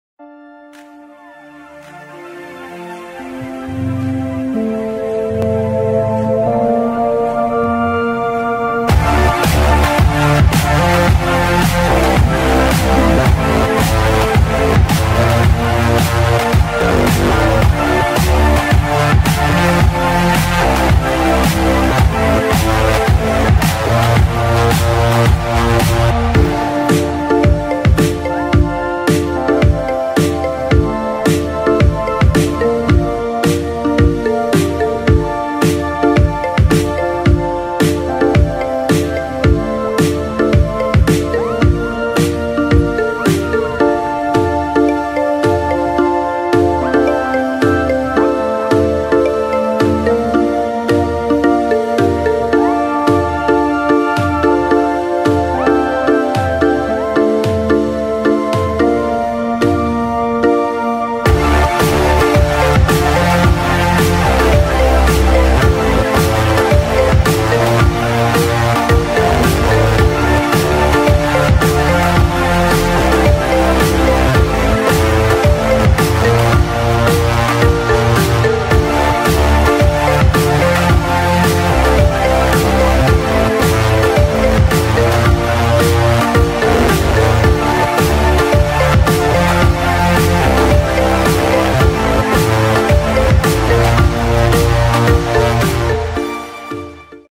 Colombia en moto. Pereira-Santarosa-Manizales-Nevado del Ruiz. Cb500x Osmo Action
Trayecto en moto Pereira-Santa Rosa-Manizales-Nevado del Ruiz.